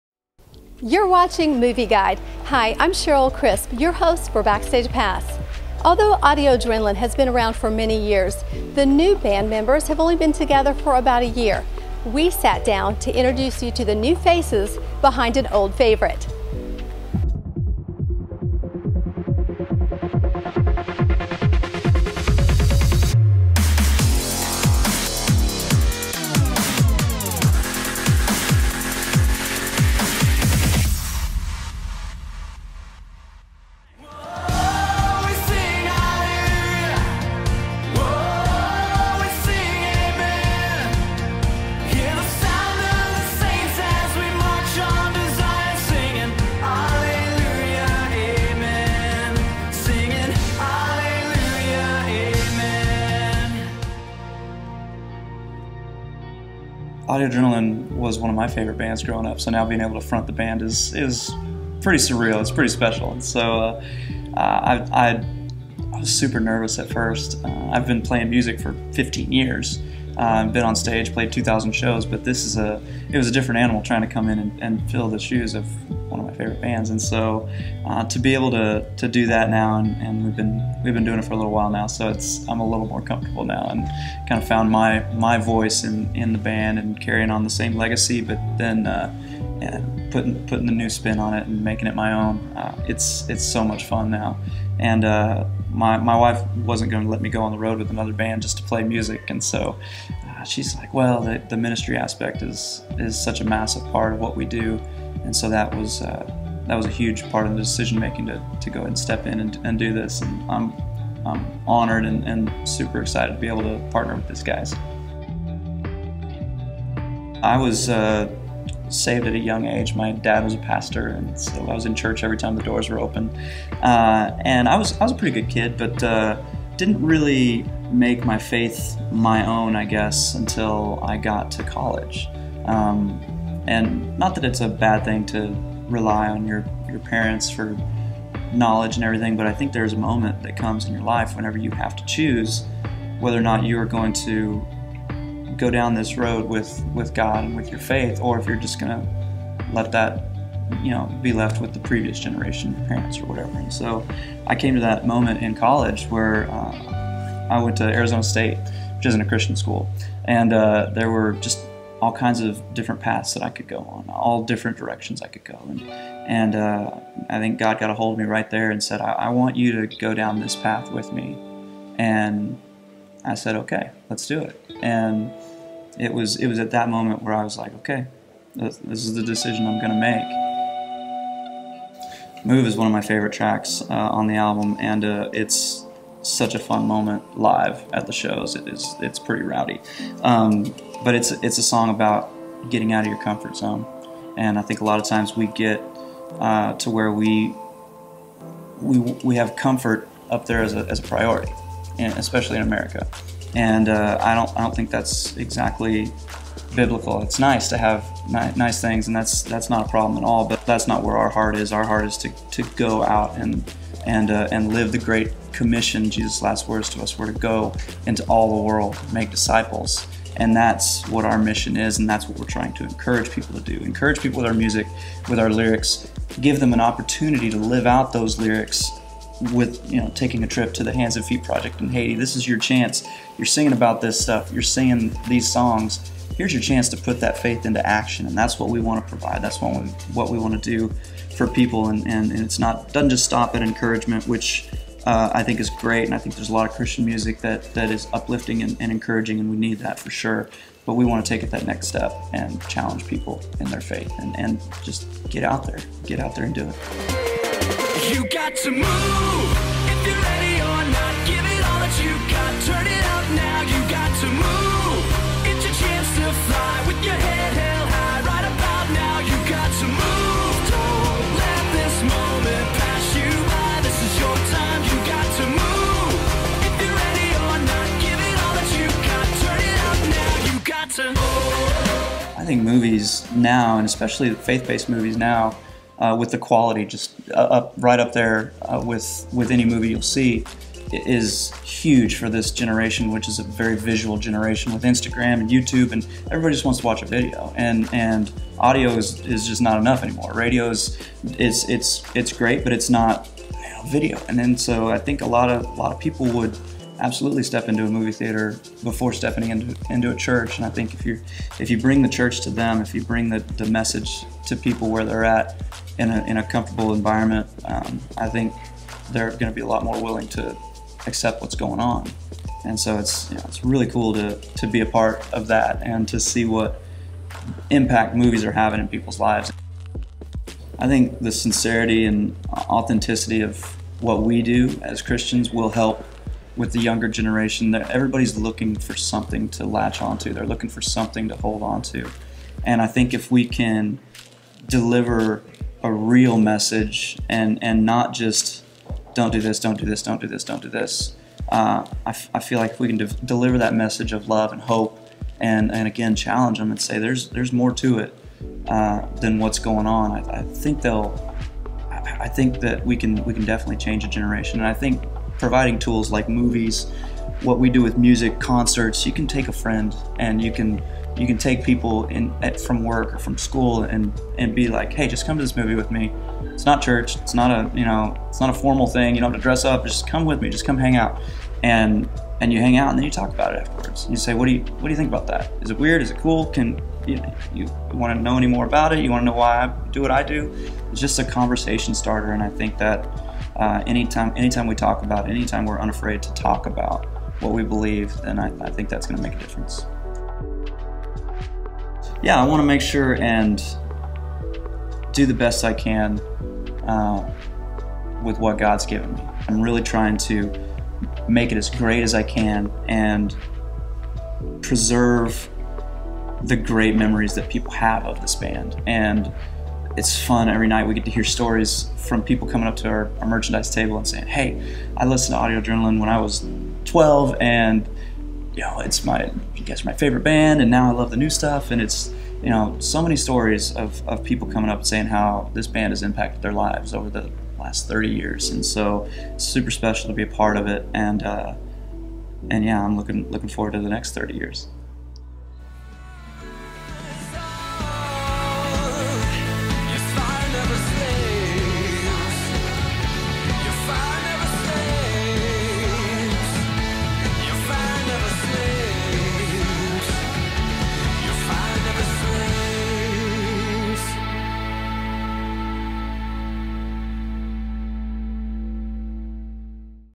Backstage Pass Ep. 9: Audio Adrenaline
There is a moment in everyone's life when you must decide whether to follow God or the world. In an Exclusive Interview, Audio Adrenaline's Lead Singer Adam Agee opens up about Finding his voice, choosing God and putting your Faith into Action.

Know Before You Go with Movieguide®!